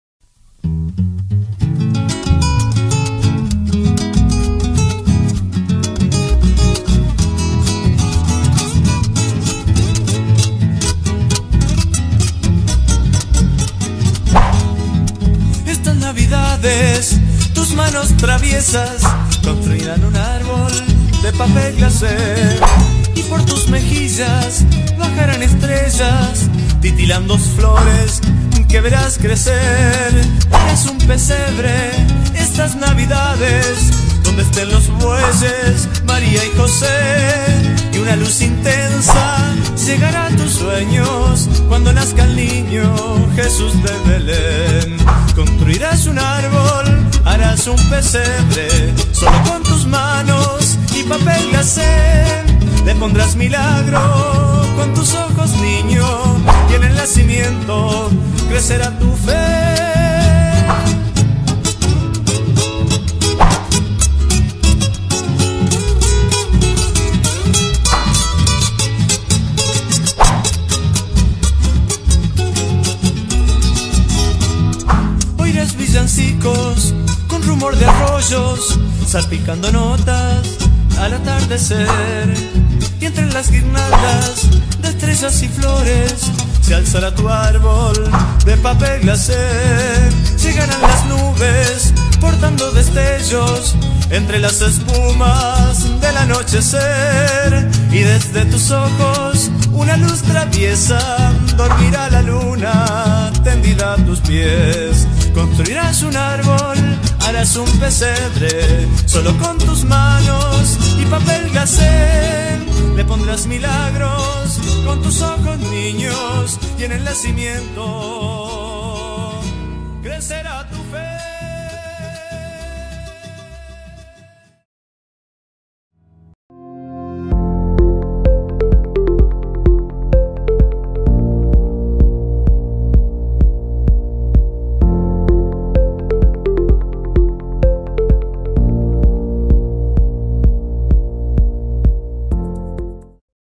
Papá Noel desde el Polo Norte para Chajarí INFO
🎅 Felices Fiestas para todos ! 🎄 Compartimos con vos un video que nos ayudó a editar Papá Noel.

Portal de Turismo y eventos de la ciudad de Chajarí Entre Ríos Argentina